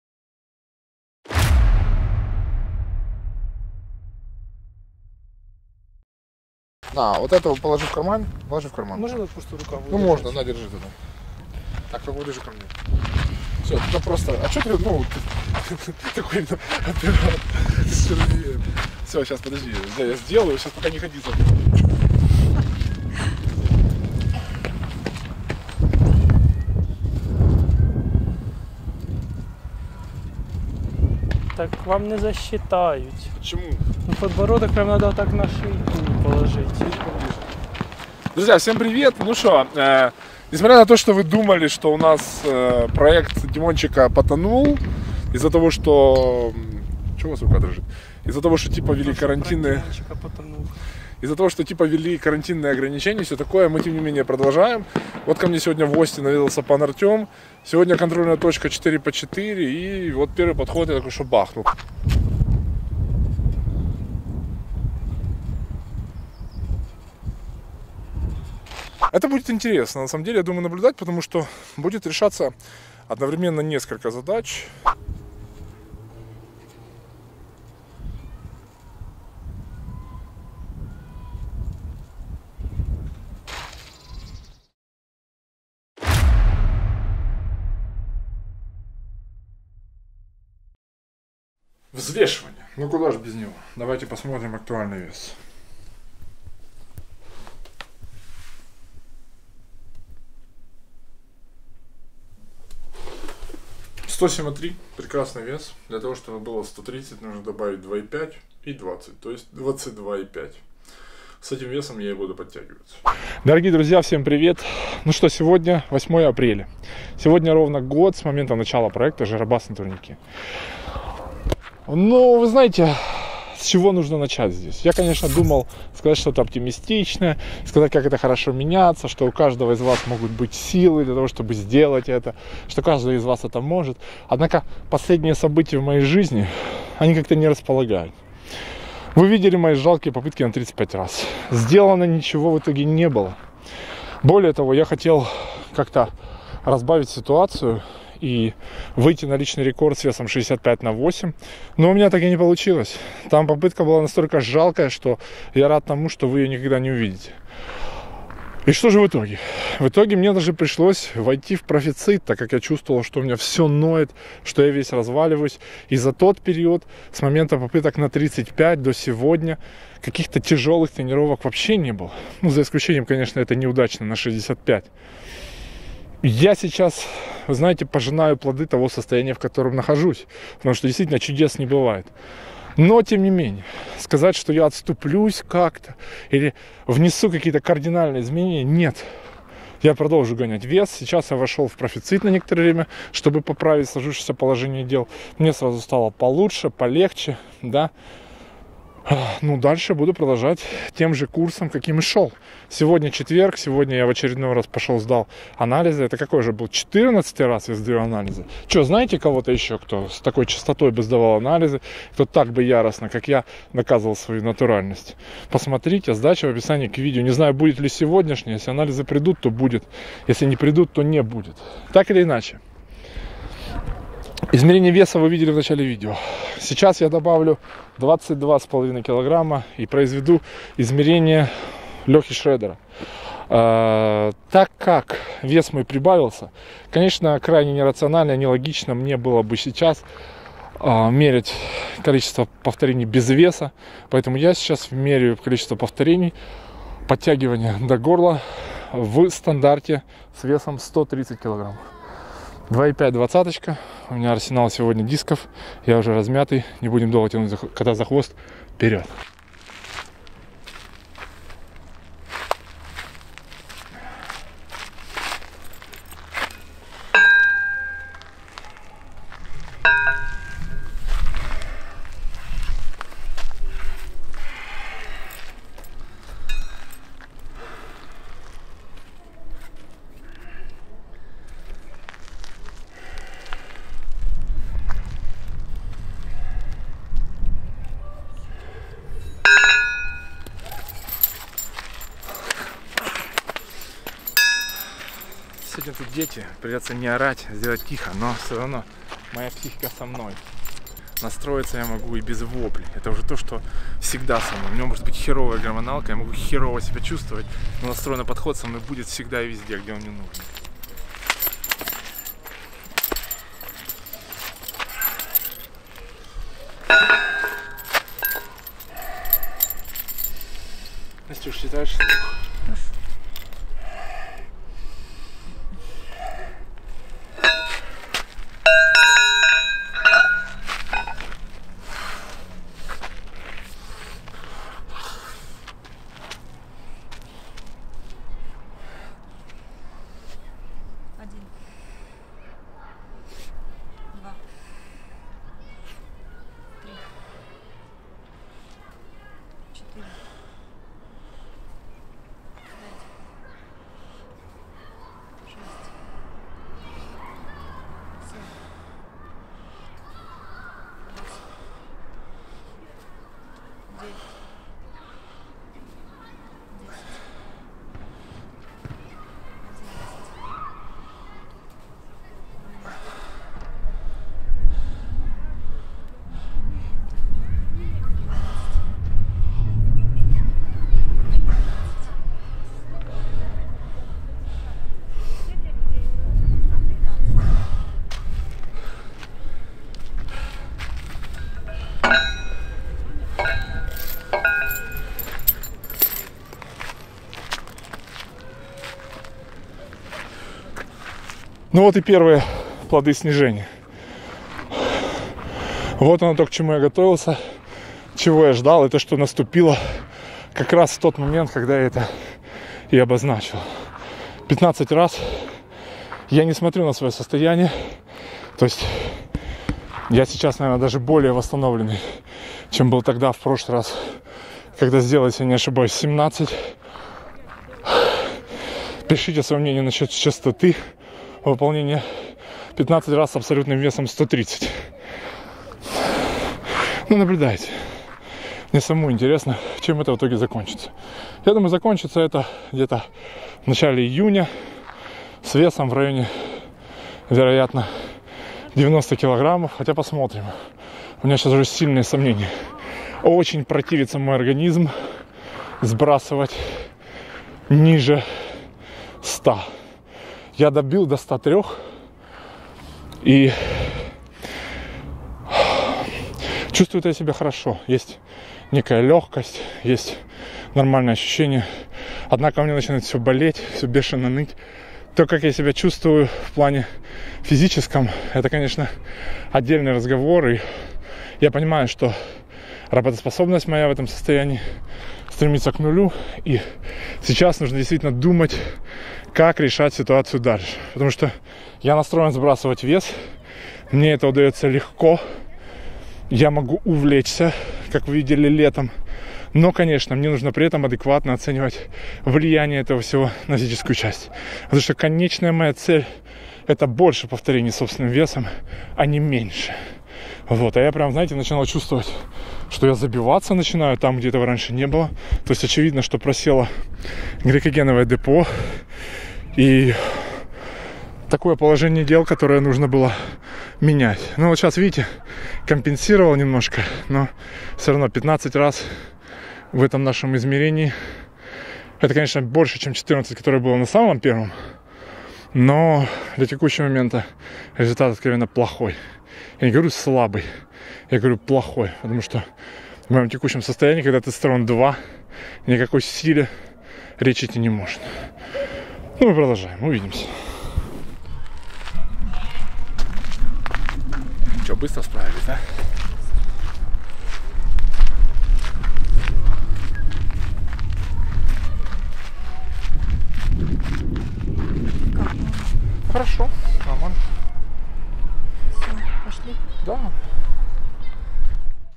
#63. Год жиробасу. Измерение Шредера. На пороге уныния
Видео начинается с отсылки к моменту, примерно год назад, во времена начала проекта "Жиробас на турнике". Тогда Димончик, еще довольно веселый и жизнерадостный, рассказывает о грядущем проекте на турниках. Подтянуться было необходимо 30 раз. Он еще не знает того, какие метаморфозы: внешние и внутренние его ожидают.

Казалось бы, год не такой значимый срок, чтобы увидеть какие-то кардинальные изменения. Однако в данной ситуации, увидеть преобразования результата и, что еще интересней, личности человека можно невооруженным глазом. Мне нравится ставить над собой эксперименты. Я понимал, что этот проект изменит меня. Однако, что изменения будут настолько существенными я тогда представить не мог.

Где-то в середине пути мне говорили, что я сильно поменялся. Стал злым, агрессивным, скрытным. Кто-то отводил это на влияние сушки. Кто-то говорил, что меня ломает внешнее давление. Хотя, мне сложно назвать это сломом. Я смотрю на себя тогда, в начале, и хочется сказать в сердцах: "А хули ты радуешься?". Да, я изменился. В числе причин скорее совокупность, а не что-то конкретное. Лично я нахожу такие изменения положительными, учитывая сложившееся положение дел. И интересно то, как эта тенденция поведет себя в дальнейшем.

Таймкоды:
00:00:00 Год назад. Снова жиробас и снова на турнике;
00:01:25 Взвешивание уже сегодня;
00:01:53 Начало ролика. Подтягивания 130 кг;
00:02:25 О попытках "разбавить ситуацию";
00:05:25 Подход подтягиваний;
00:06:40 Итоги, выводы.

В видео приводится контрольный подход, который характеризует измерение в обозначенный ранее Алексеем Шредером манер. Суть в том, что атлет должен подтянуться с совокупным весом 130 килограммов максимальное количество раз.
Проект "Искупление" продолжается. Будем искупать, пока не искупим.